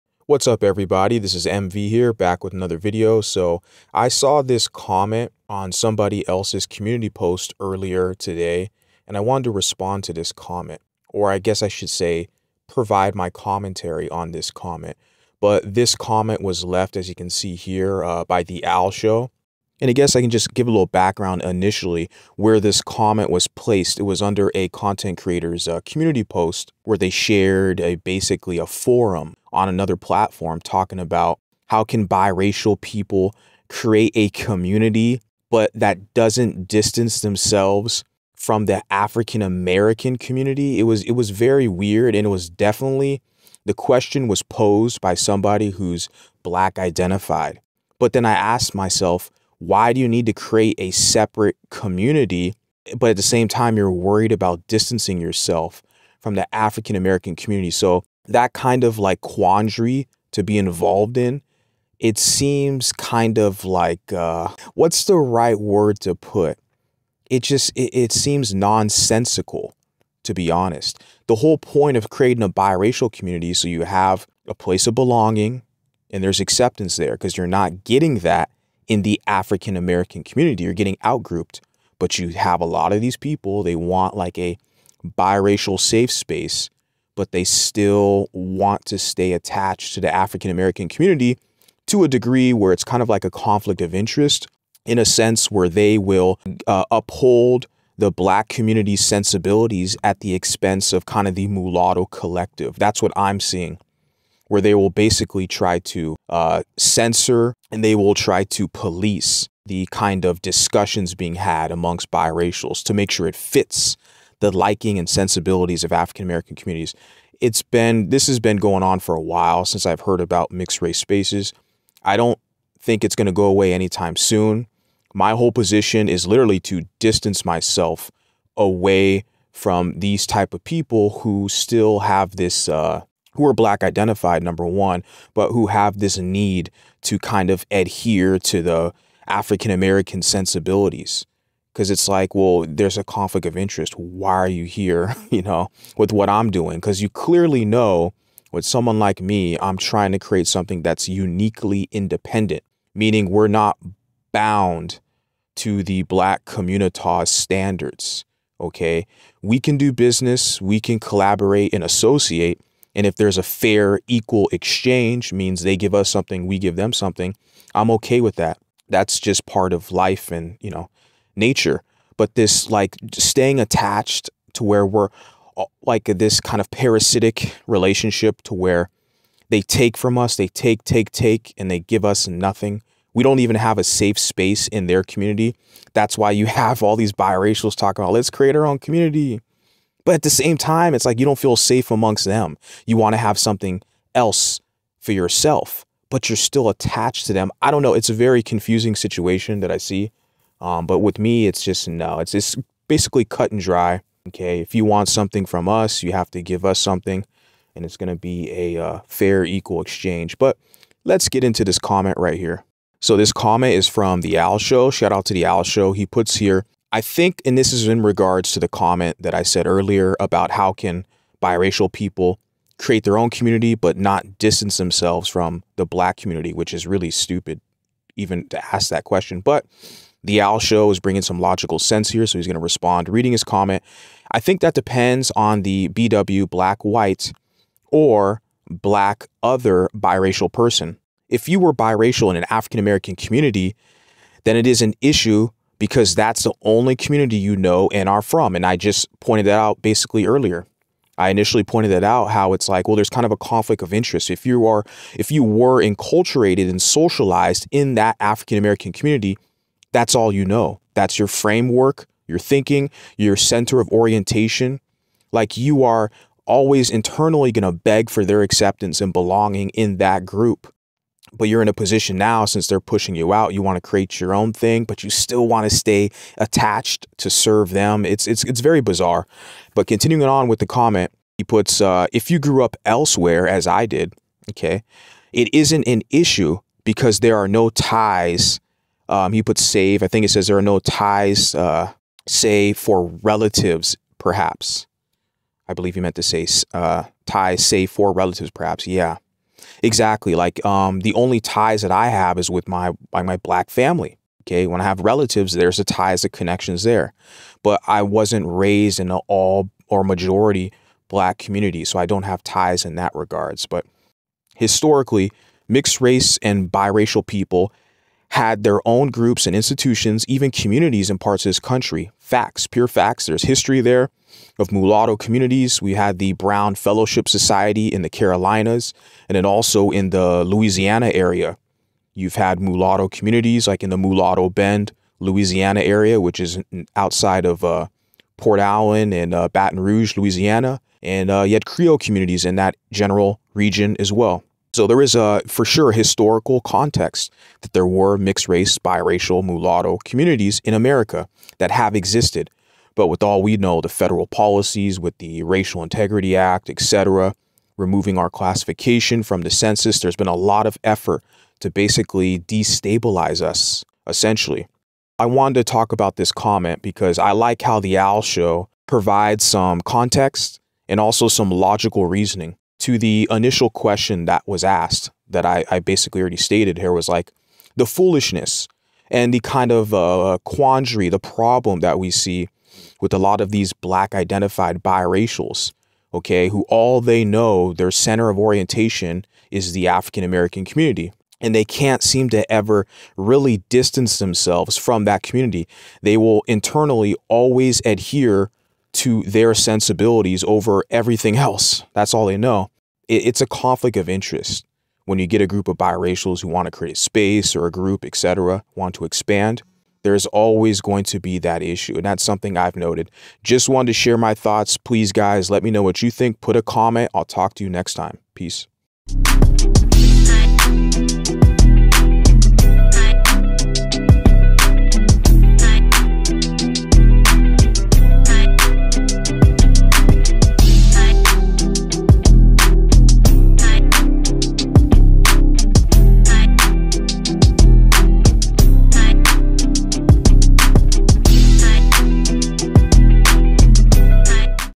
The Black-Identified Biracial Quandary
They refuse to let go. Even after many Biracial Wake Up Calls they will still adhere to black sensibilities at the expense of the Biracial collective.